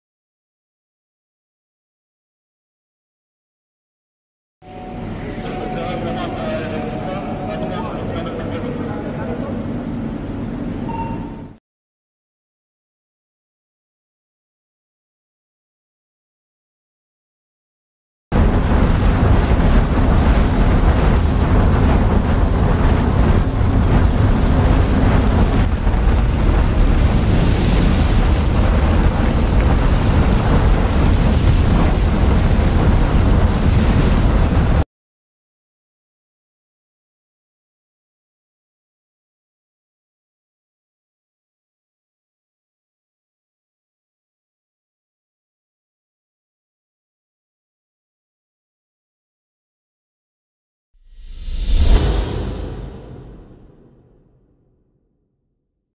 התלקחות סוללת ליתיום
בסרטון ניתן לראות כיצד סוללה של טרקטורון חשמלי התלקחה וכיצד ניתן לפעול בזהירות וביעילות למניעת אסון כבד לרכוש ונפש.
חברת ש.ה גורן מתמחה במתן פתרונות ייעודים להתמודדות ומניעת שריפות שנגרמות כתוצאה מהתלקחות סוללות ליתיום.